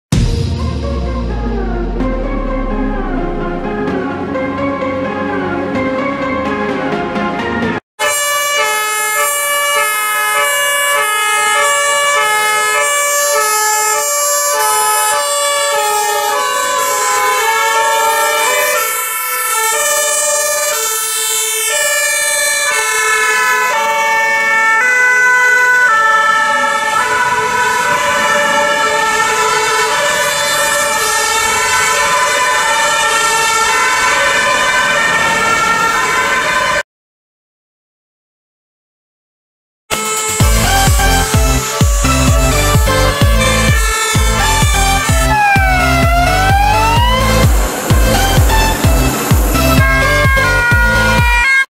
Brandvertrek brandweer Brussel Helihavenlaan / Départ incendie pompiers Bruxelles!
Brandvertrek met commandowagen R58 , ladderwagen E11 , autopomp P16 en ambulance A43.